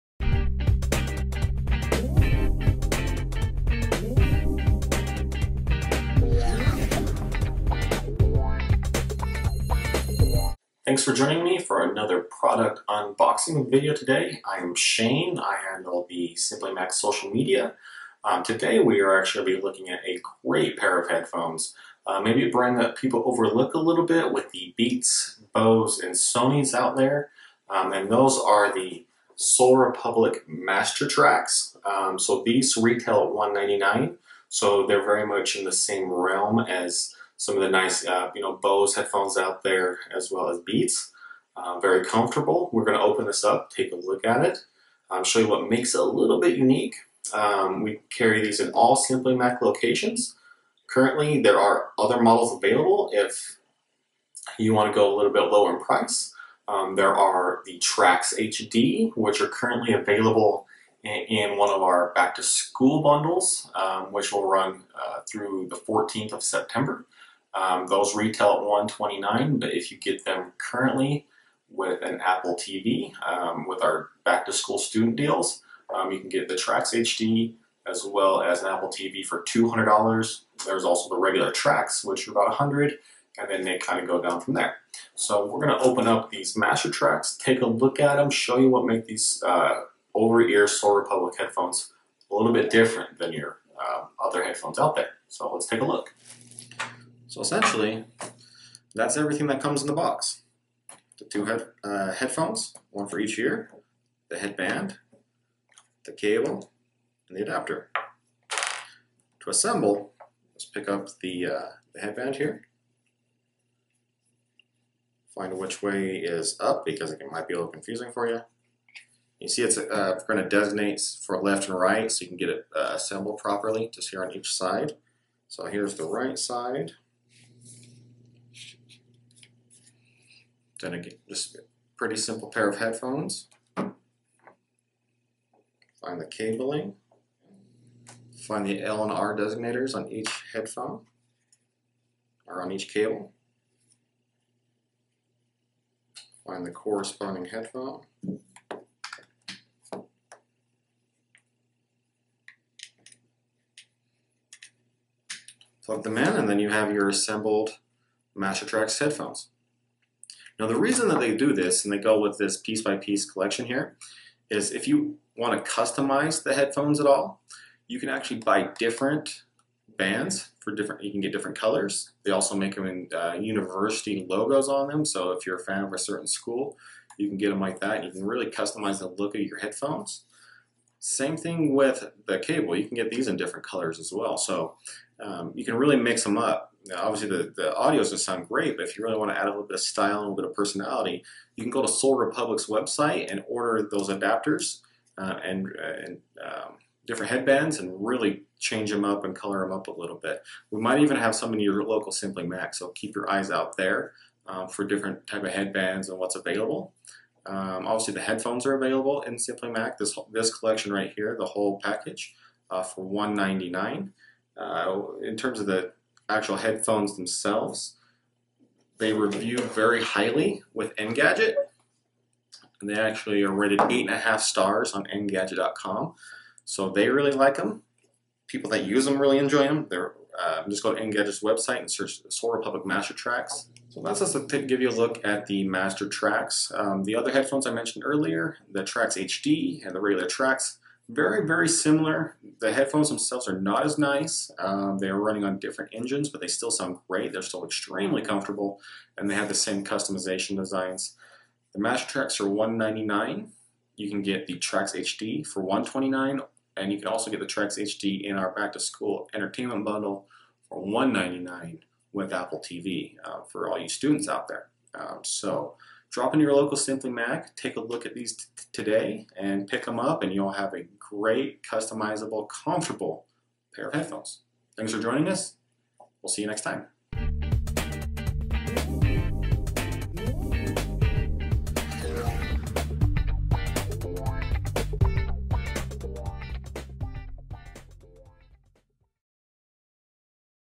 Accessory Spotlight: Sol Republic Master Tracks
This video is about the Sol Republic Master Tracks, great and comfortable headphones.